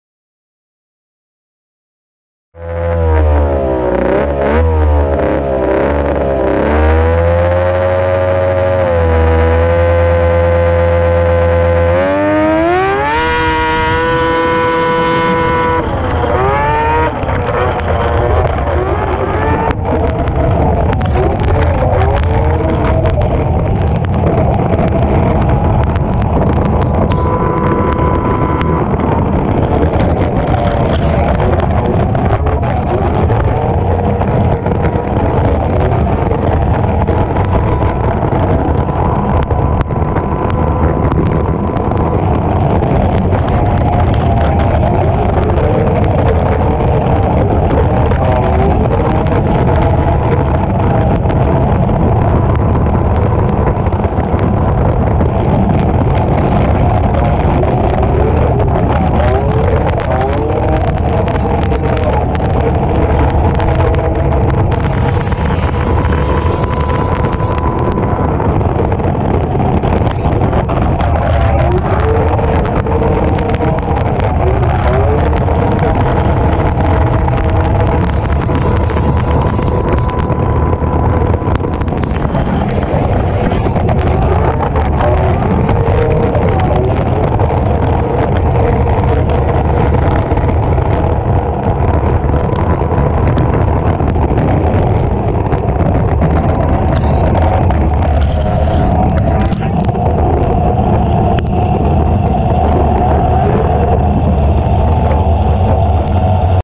ON BOARD: 2022 Open 600 Heat - #22 Austin Leeck
GoPro mounted to the trailing arm? Sure thing!!!